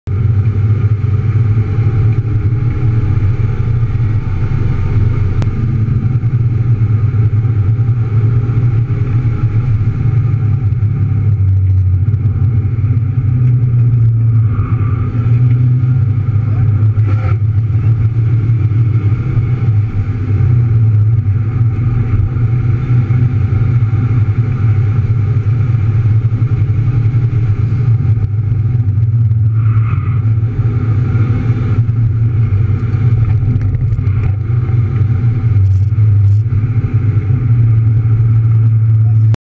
Mazda mx5 turbo 1.8 nb - nice pass trough langhe track in balocco
A nice section of the langhe track in balocco with miata turbo 1.8 nb